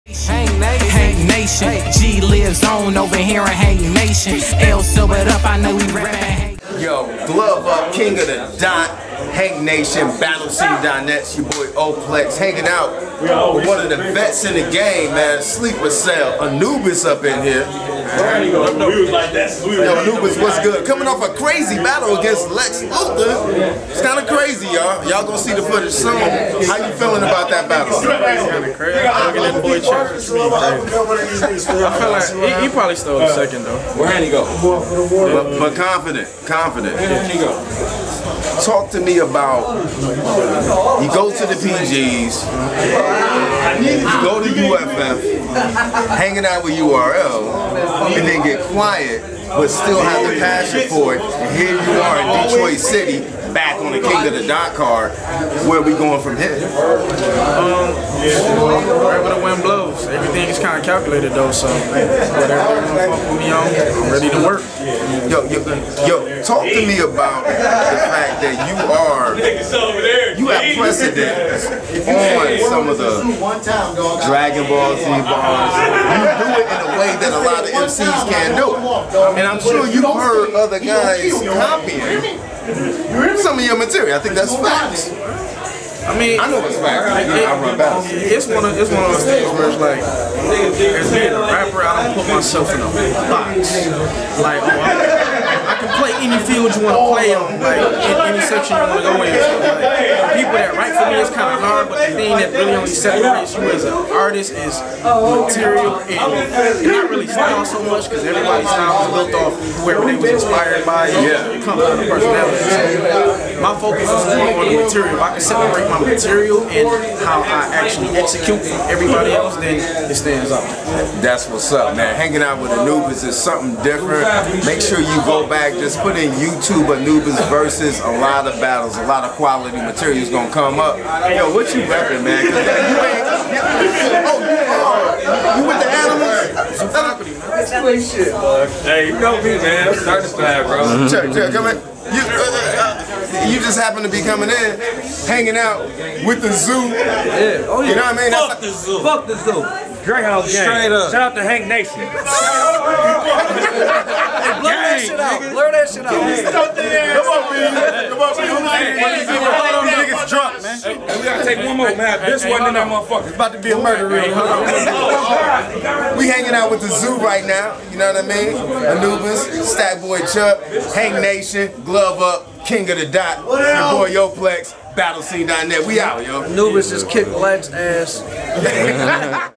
Anubis- Glove up- The Verdict Corner
HankNationTV & Battlescene.net caught up with anubis after his fire battle with lexx luthor at kotd gz event..